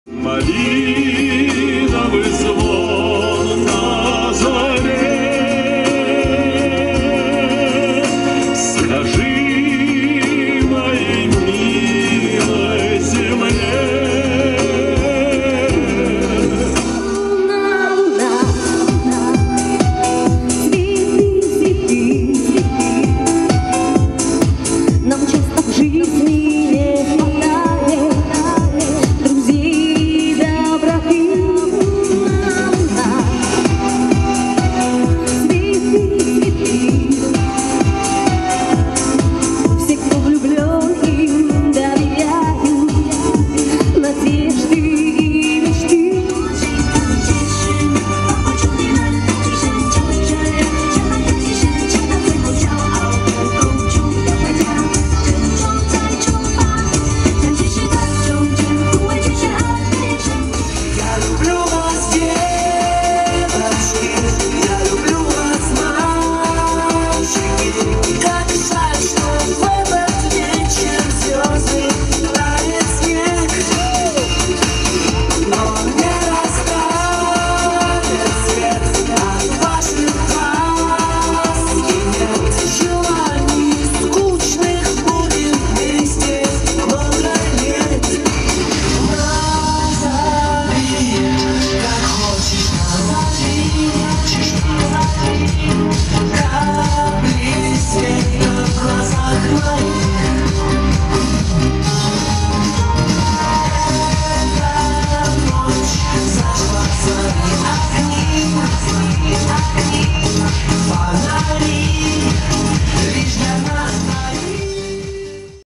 Выступление Романа Жукова
День рождения Благовещенска Ссылка на сайт / Видео предоставлено ТК "Альфа канал"